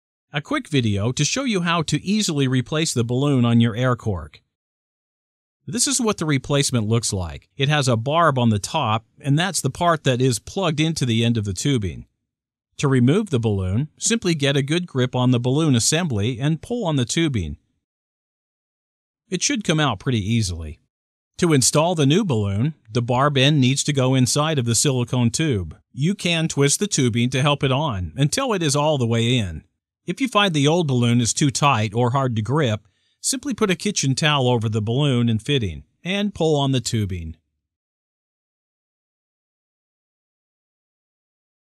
Balloon Replacement - How To
The Balloons are not meant to last forever, but are quite durable.  Here is a quick video to show how to replace the balloon.